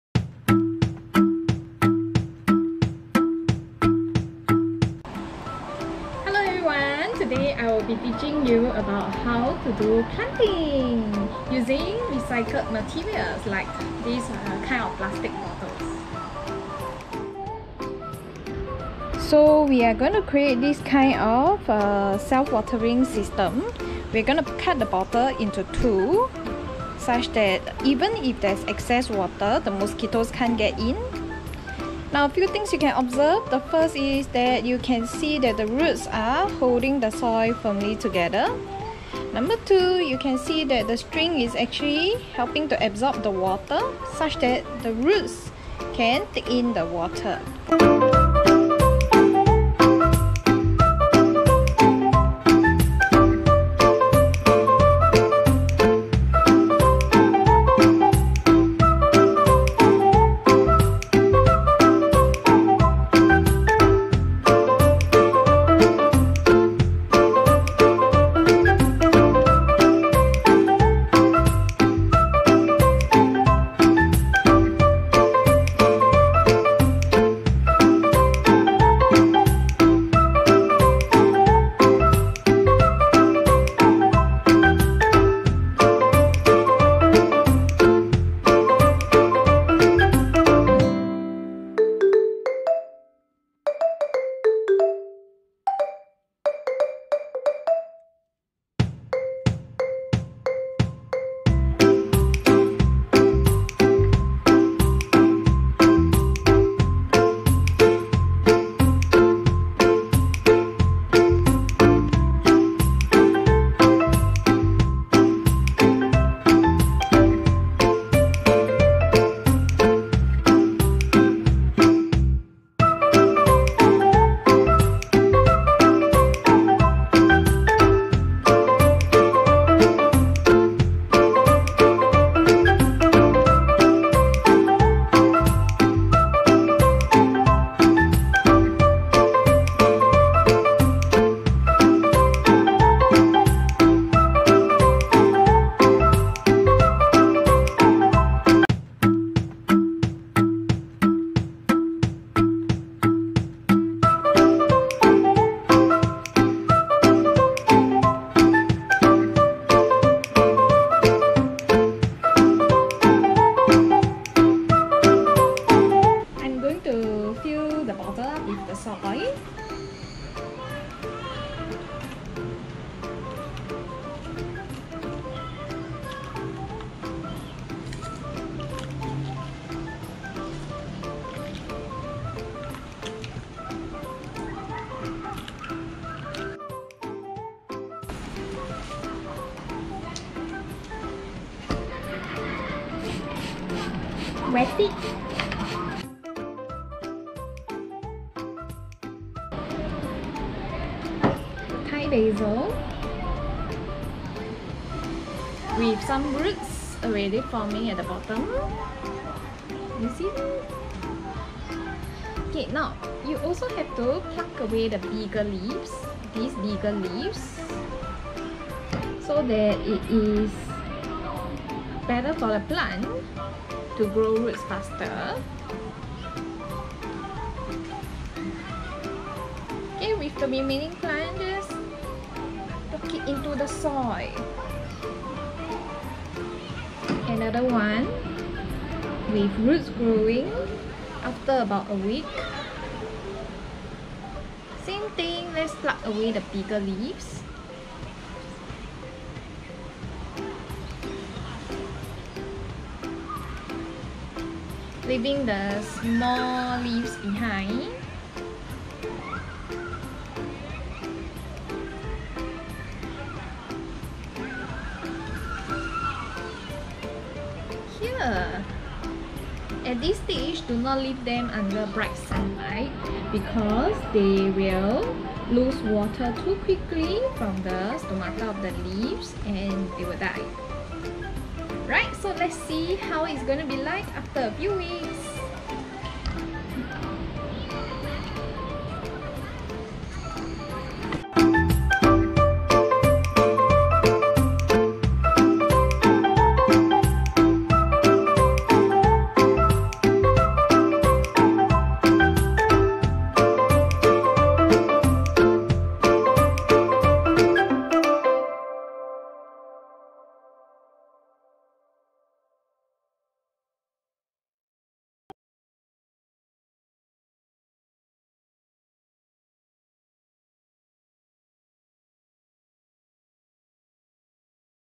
PSLE Science Vlog - Self Watering System for Plants
Extremely useful for beginners who wants a fuss free planting system.

Learn about the science behind the whole process of planting! :)